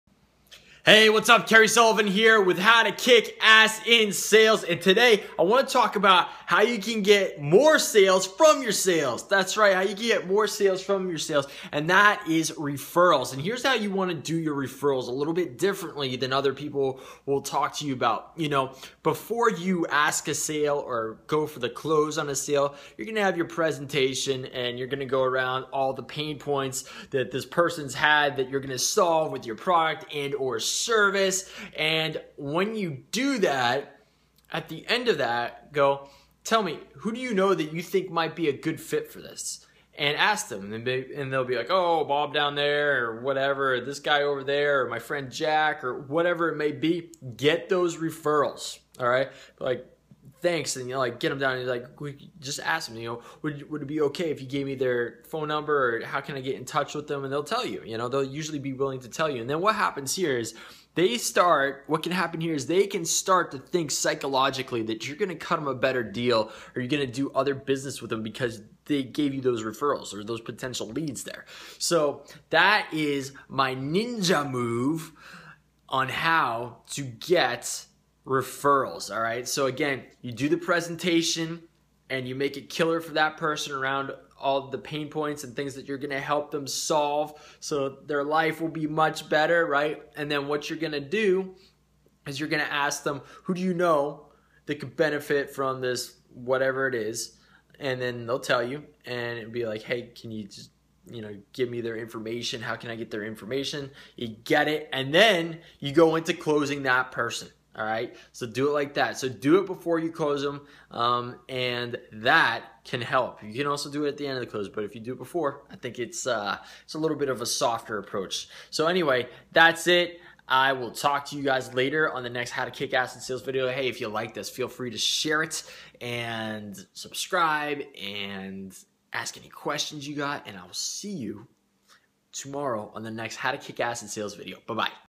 How to kick ass in sales #45 how to get more sales ....from your sales
Killer way to get referrals before you close someone